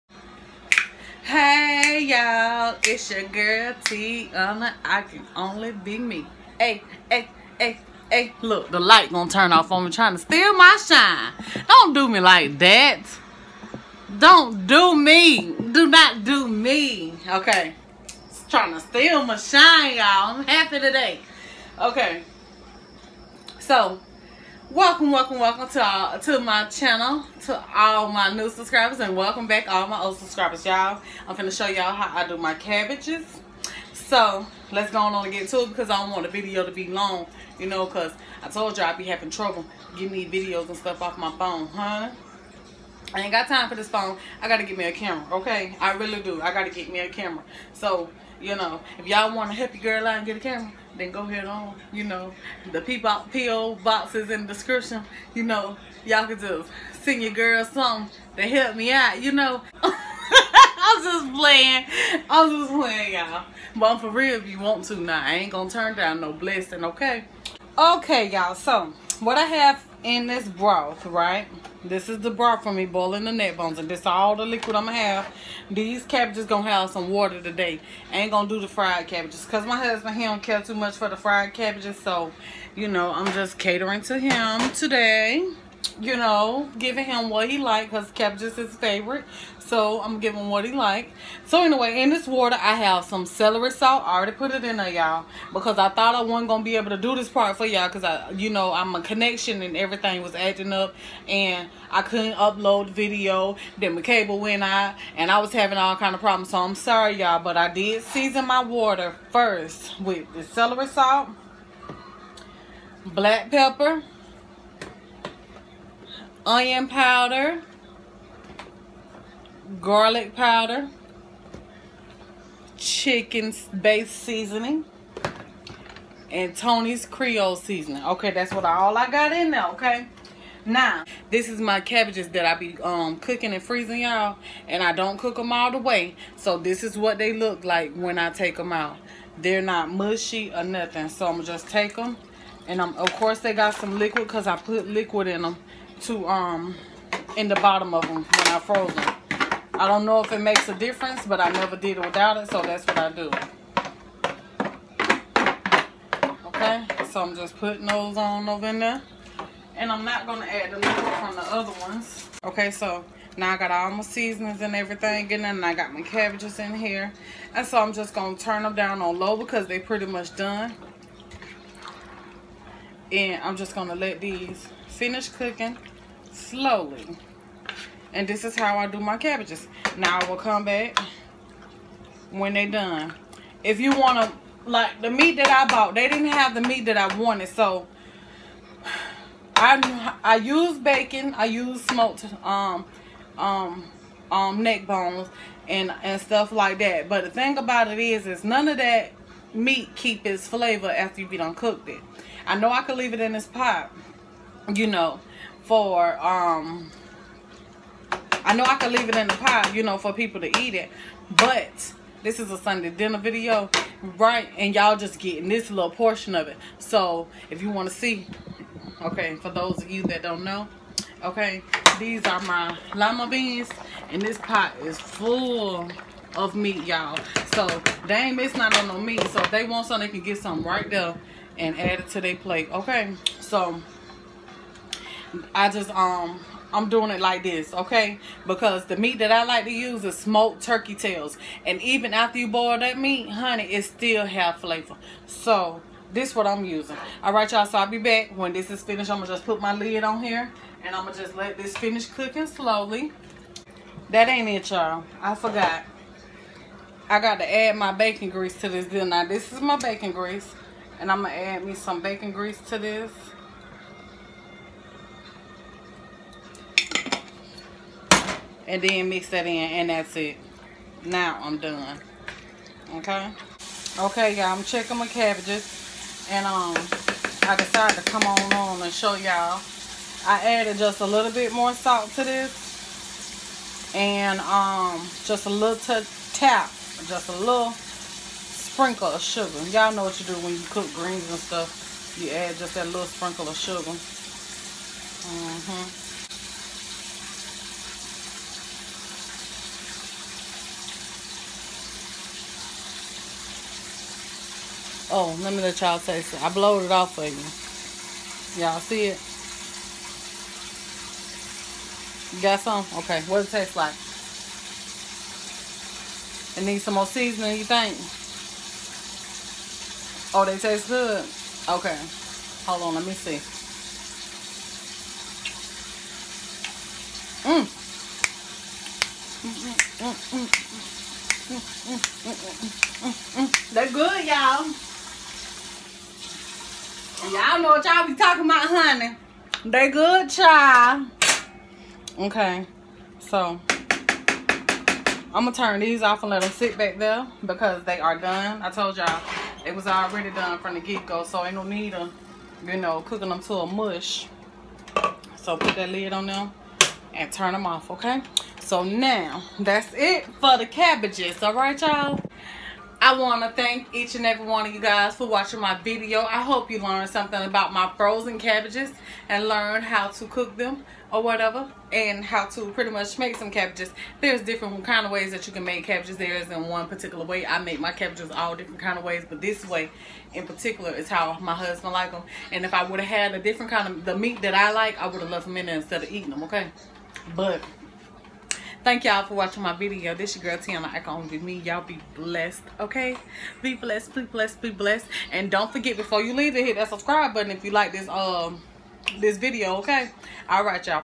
THE BEST SIMPLE AND DELICIOUS CABBAGE
HEYY YALL I WILL BE SHOWING YOU STEP BY STEP HOW I MAKE MY DELICIOUS CABBAGE. THAT I PREPARED AHEAD OF TIME AND FROZE. I HOPE YALL ARE HAVING A WONDERFUL DAY. PEACE AND BLESSING TO ALL OF YOU!!!

FACEBOOK: BE YOU TIANA ICANONLYBEME